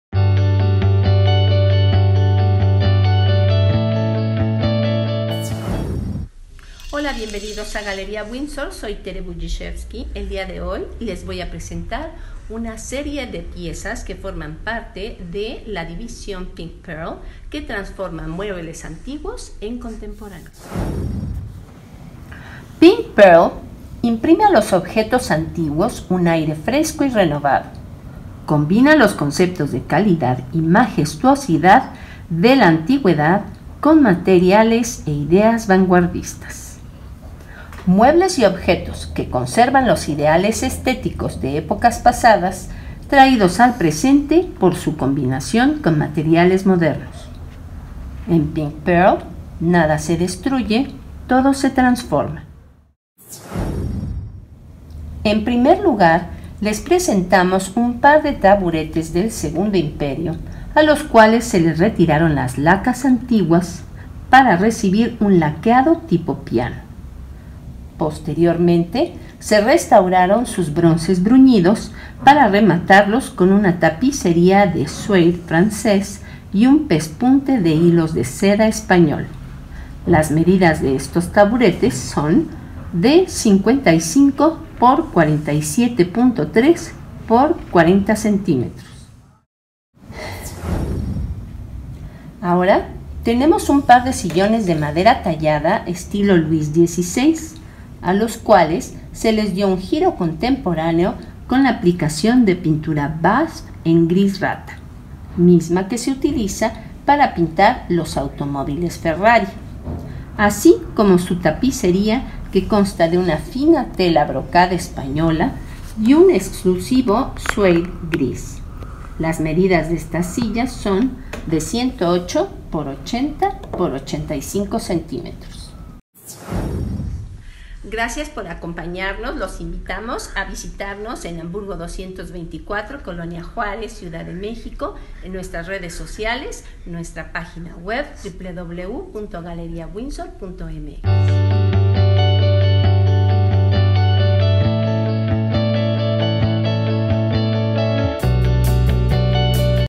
Muebles Pink Pearl
PINK PEARL MR imprime a los objetos antiguos un aire fresco y renovado; combina los conceptos de calidad y majestuosidad de la antigüedad con materiales e ideas vanguardistas. Muebles que conservan los ideales estéticos de épocas pasadas traídos al presente por su combinación con materiales modernos.